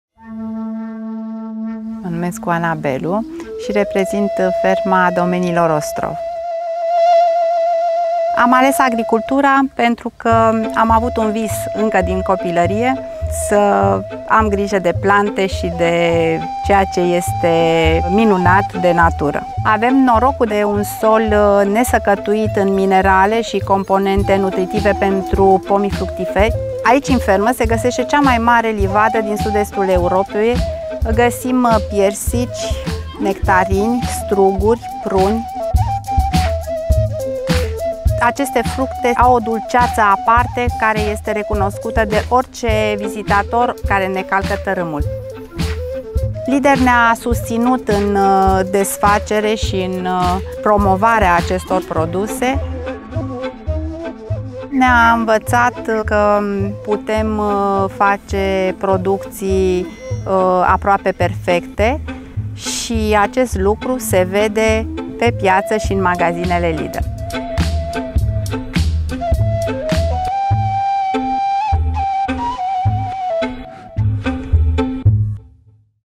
Producatorul Hortifruct 1 • Piata Lidl
Pentru a ne asigura ca misiunea noastra e bine indeplinita, mergem noi insine la fermele partenere. Urmarim cu atentie transportul si depozitarea fructelor si legumelor.
Pe scurt, supervizam drumul pe care produsele il parcurg astfel ele sa ajunga la tine proaspete, asa cum trebuie sa fie!